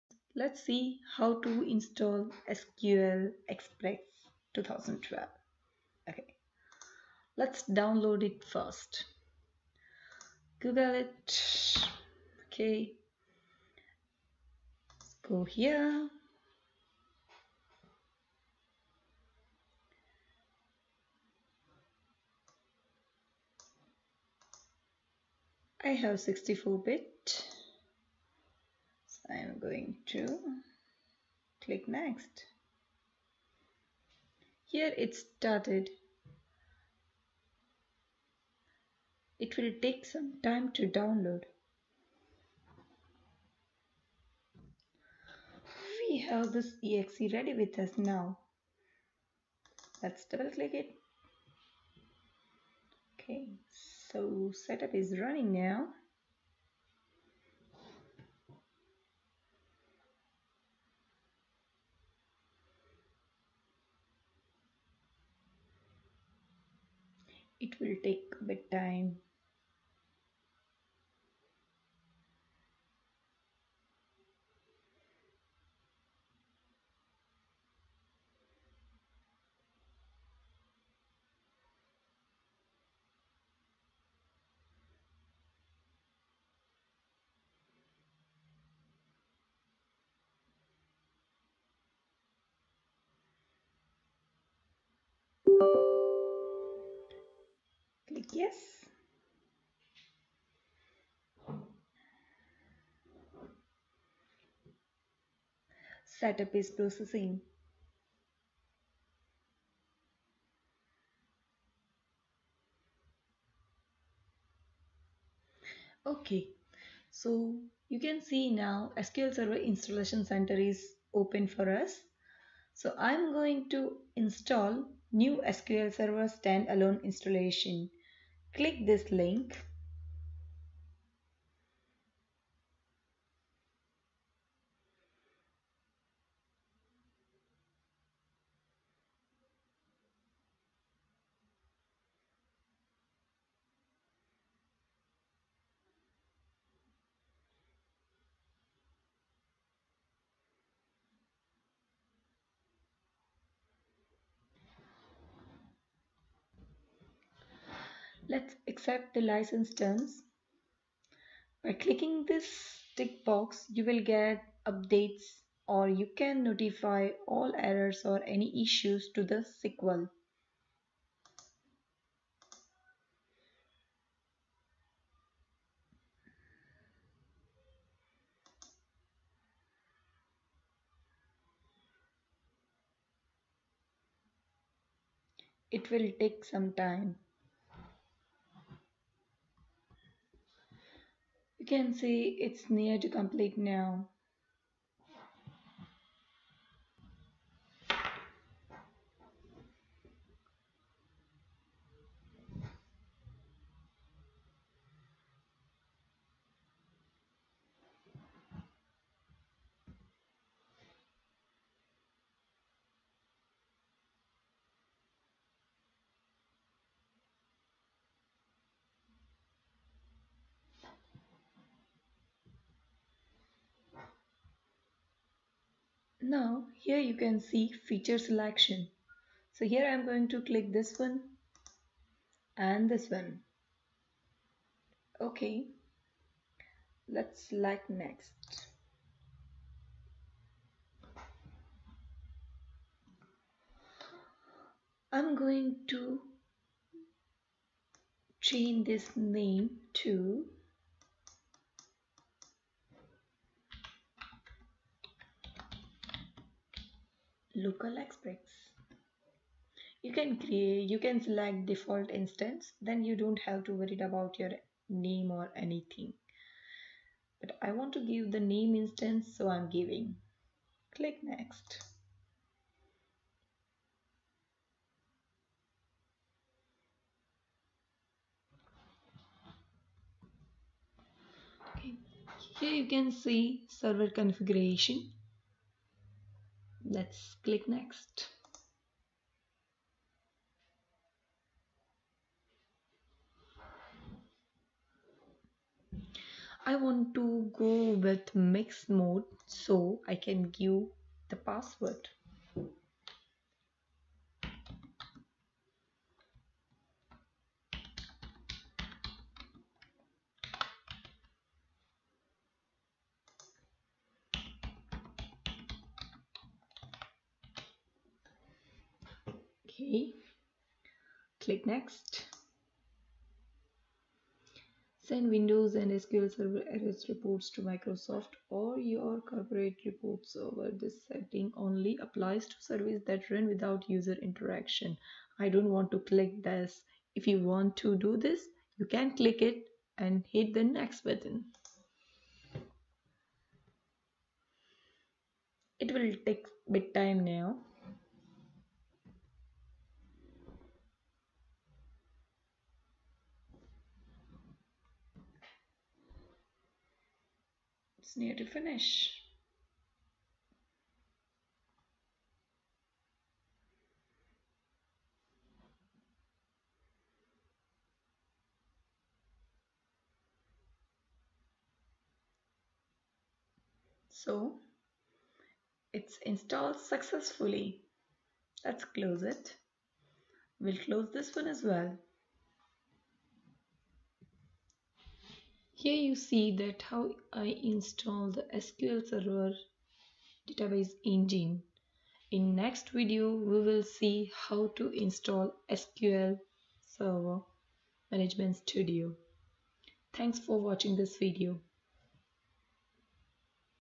Install SQL Server Express 2012 - PART 1 | Install SQL SERVER DATABASE ENGINE | Create SQL Instance
Hello Friends,
Here you can find how to Download SQL Server Express and Installation Process. In this Part 1 I included Installation  of SQL database engine to create SQL instance and In Part 2 Video you can find How to install SQL management Studio.